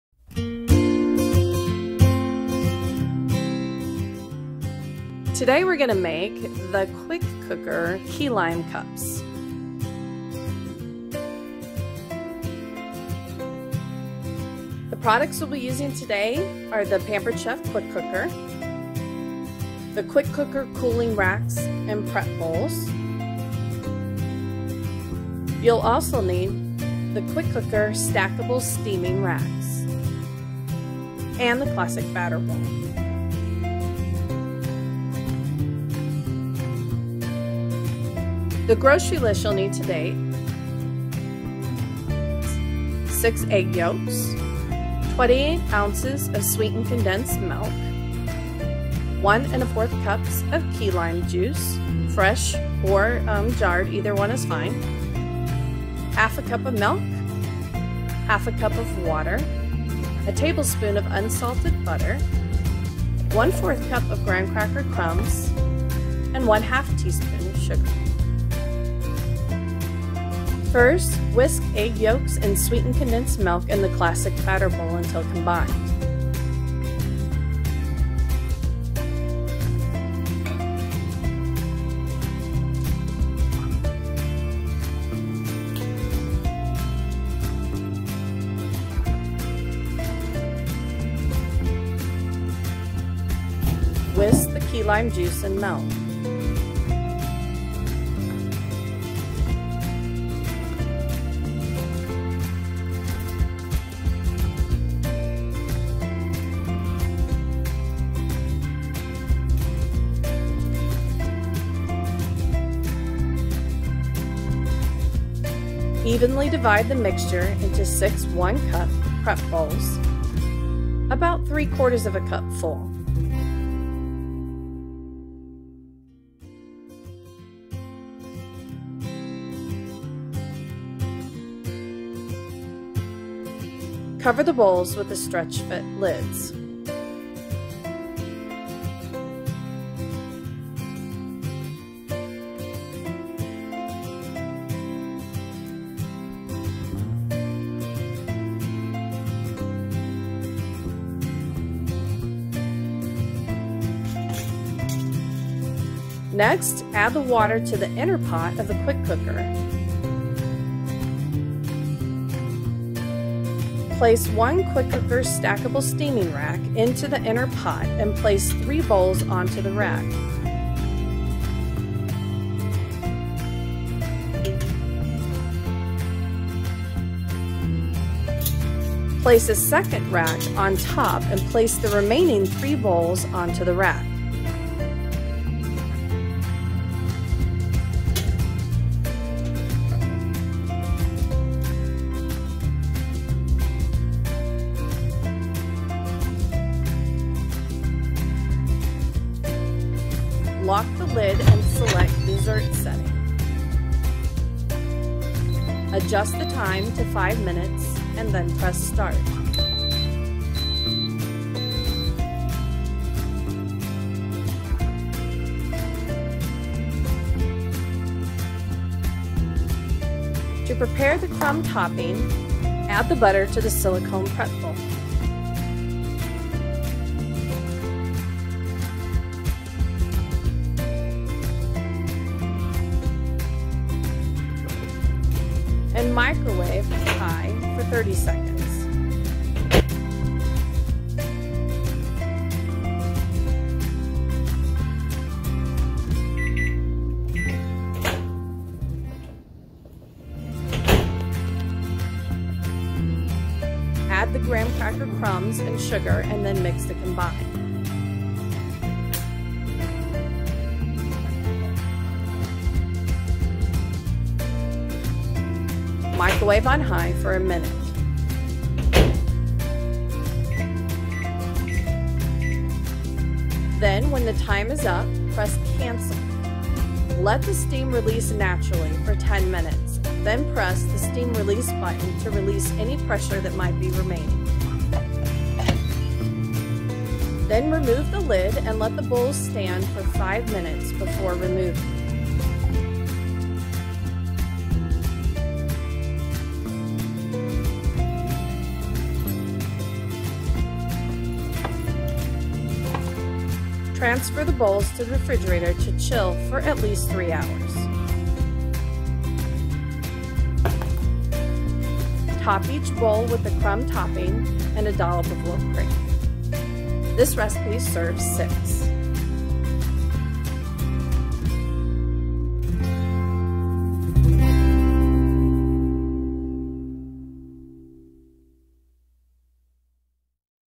How to make QUICK COOKER KEY LIME CUPS
In this video I will show you how to make SIX INDIVIDUAL SERVINGS OF KEY LIME PIE IN 20 MINUTES using Pampered Chef products!